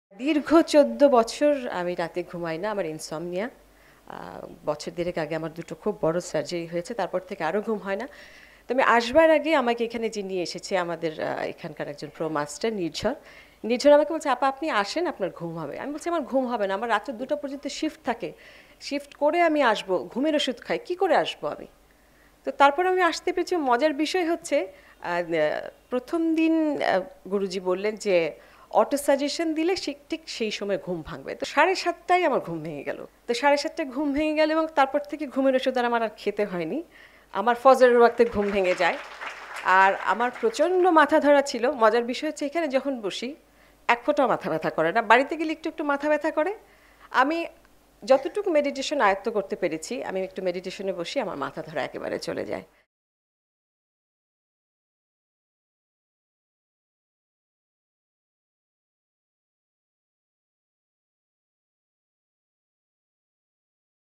Peaceful Sleep after 14 Years -RJ Sharmeen (Official)
Gazi Sharmeen Ahmed, popularly known as RJ Sharmeen, has been a prominent name in the FM radio stations in Bangladesh.

She is also a graduate of Quantum Method Course.

In this video she has shared that the meditation course and meditation practice has enabled her to sleep without any medications!